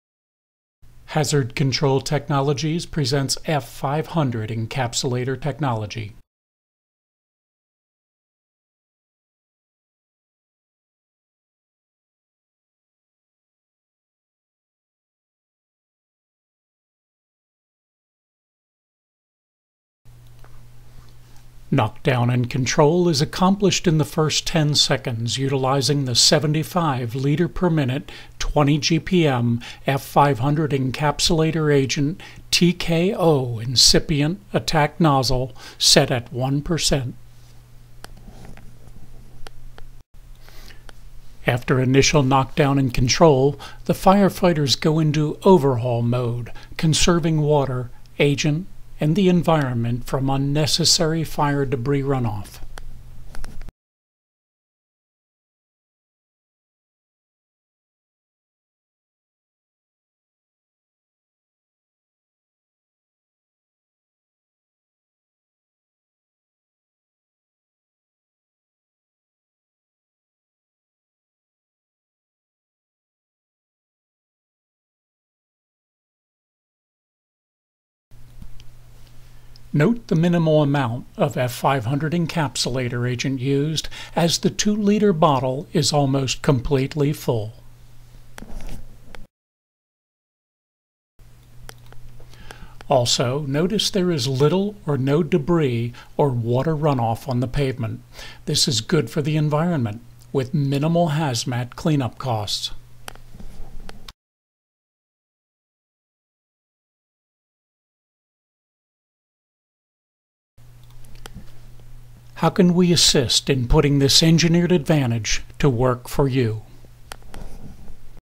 F-500 EA TKO Nozzle Extinguishes Car Fire in 10 Seconds!
F-500 Encapsulator Agent extinguishes this car fire in 10 seconds using very little F-500 EA and water.